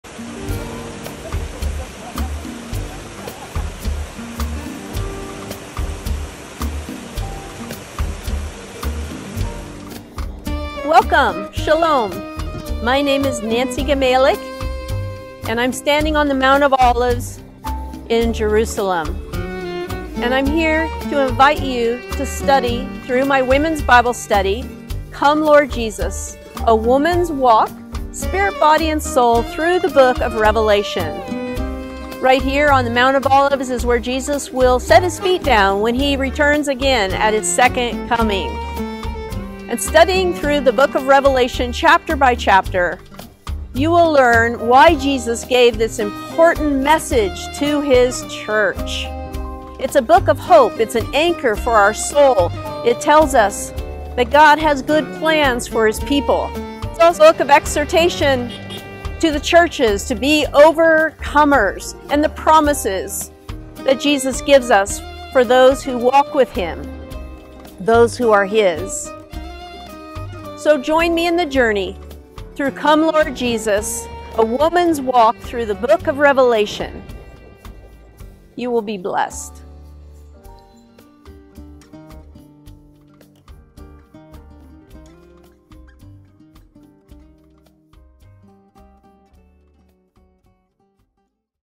Come Lord Jesus Trailer HD 1080p
This is a trailer for the Come Lord Jesus: A Woman's Walk—Spirit, Body & Soul—Through the Book of Revelation study.